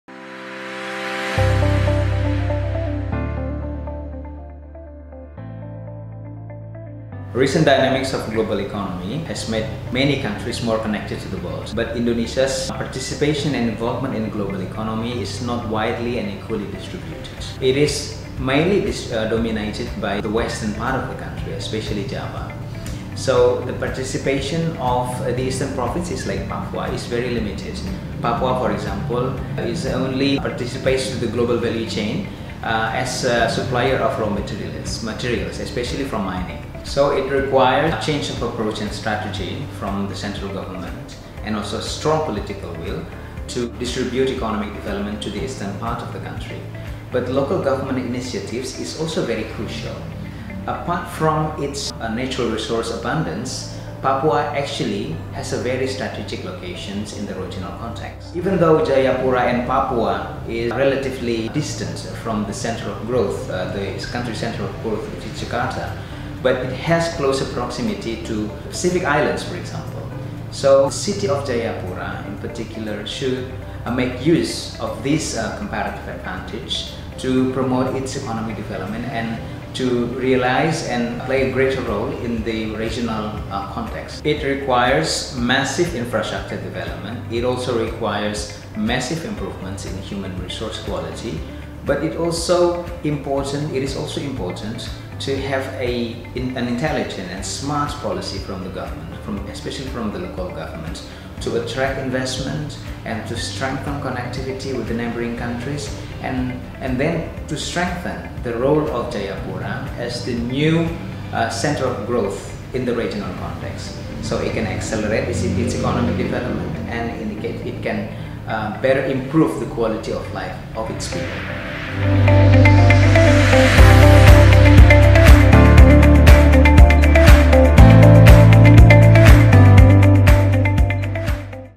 CORE INSIGHT- Jayapura as a Competitive Border City and The Regional Growth Centre
CORE INSIGHT

In this video explain how to fostering Jayapura, one of city in Papua, as a competitive border city and also the regional growth Centre.
Presented by Executive Director of CORE Indonesia, Mohammad Faisal, Ph,D

Jakarta, 22 April 2019